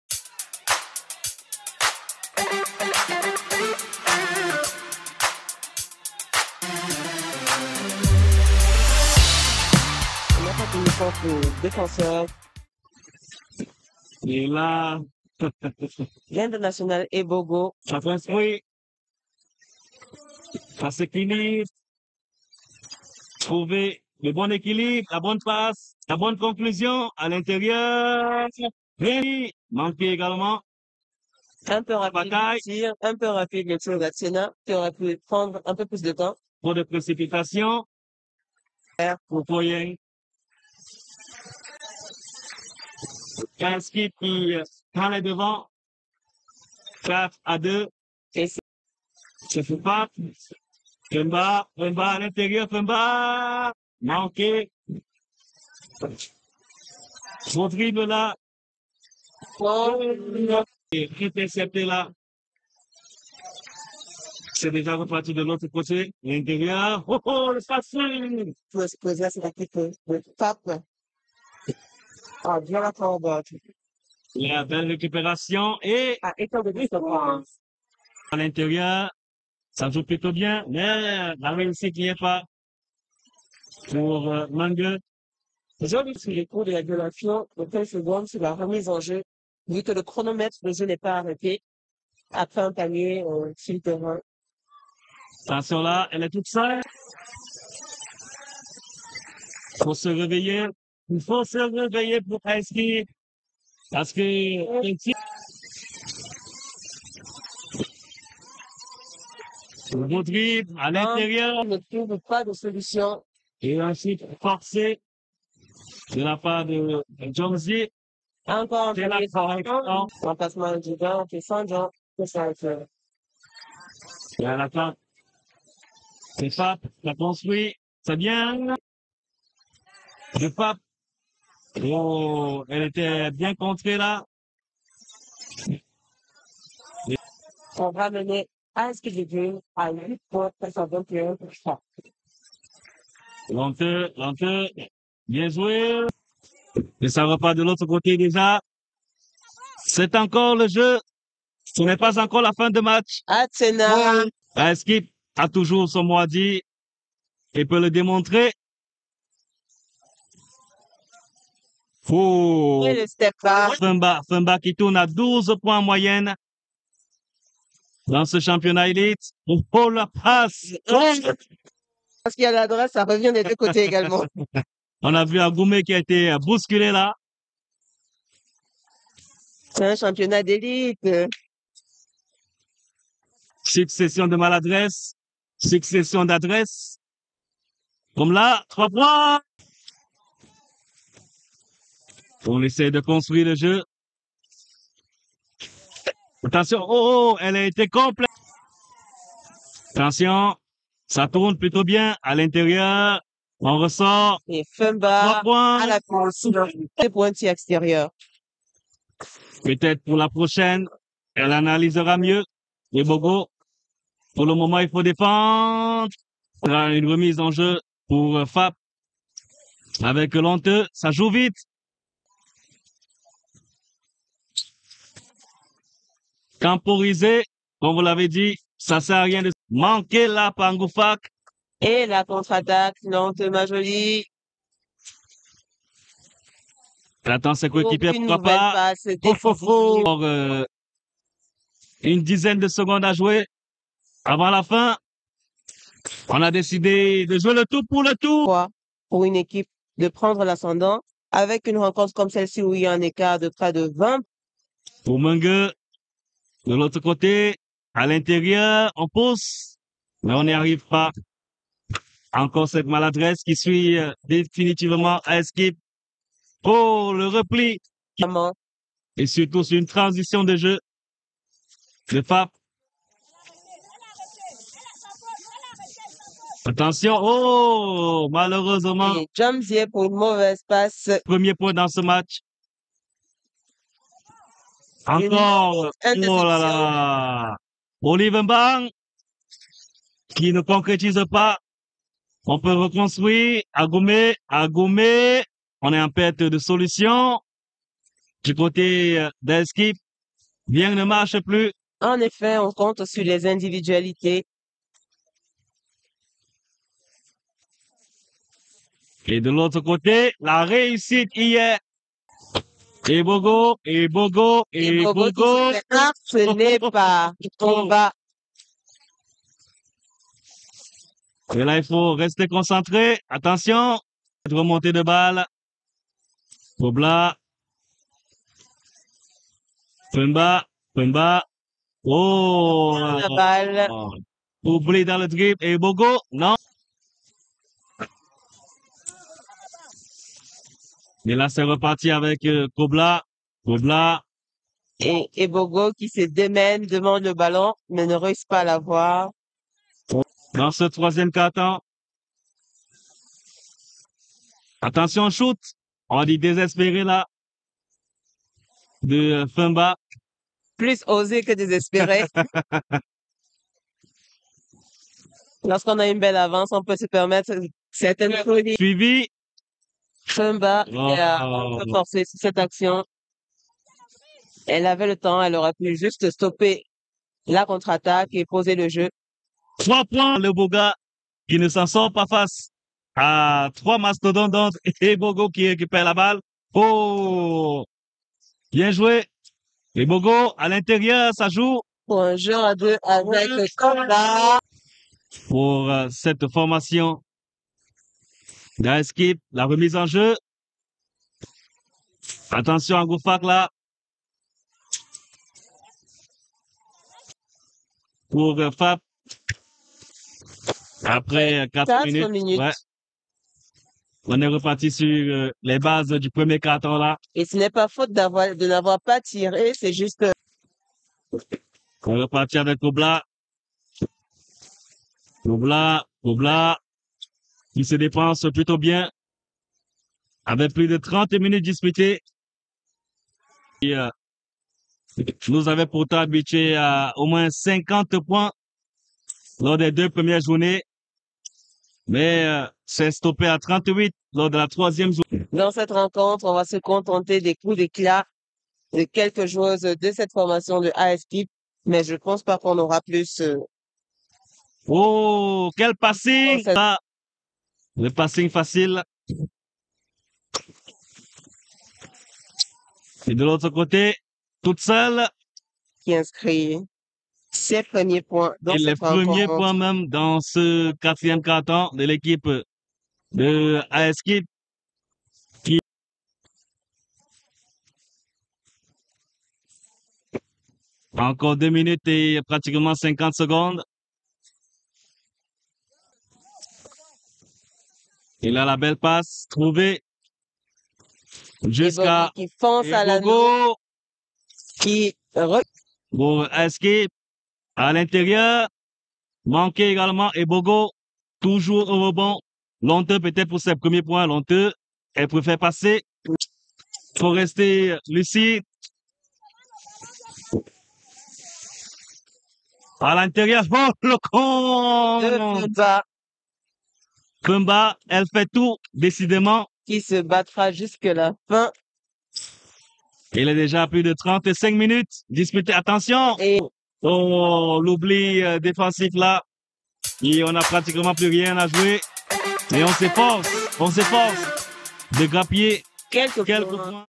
BASKETBALL CAMEROON - ELITE SENIORS DAMES 2024 - 4è JOURNEE | FAP vs AS KEEP
Match Elite seniors Dames opposant FAP Basketball à AS KEEP THE DREAM comptant pour la 4è Journée.
Victoire des FAP avec une jolie performance de la jeune pepite JOSIANE FEUMBA.

Crédit images : @fecabasketofficiel1201 , @africa24sport